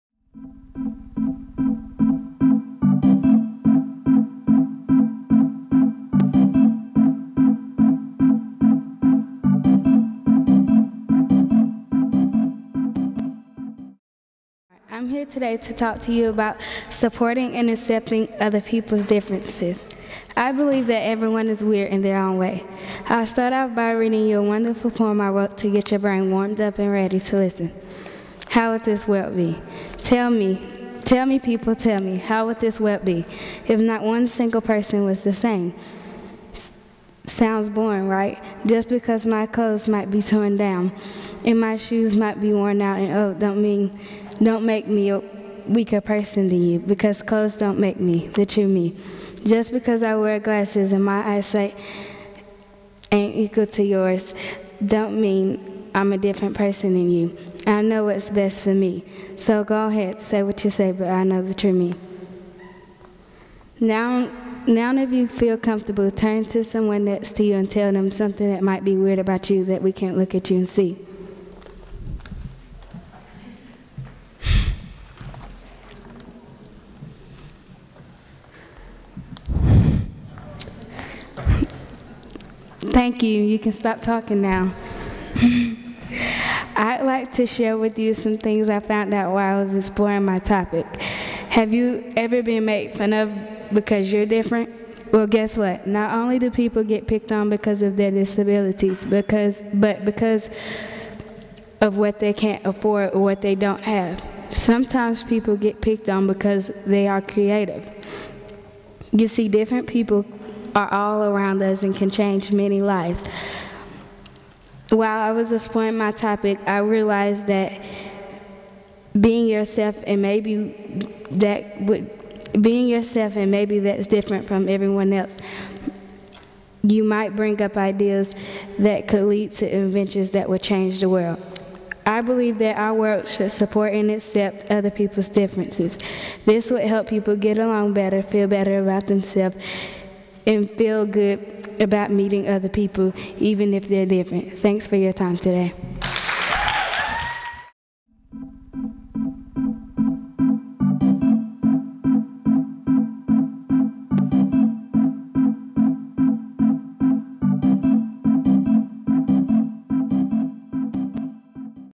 Accepting Differences: Shawntelle at TEDxYouth@IsaacDickson
Shawntelle speaks about how everyone is weird in their own way.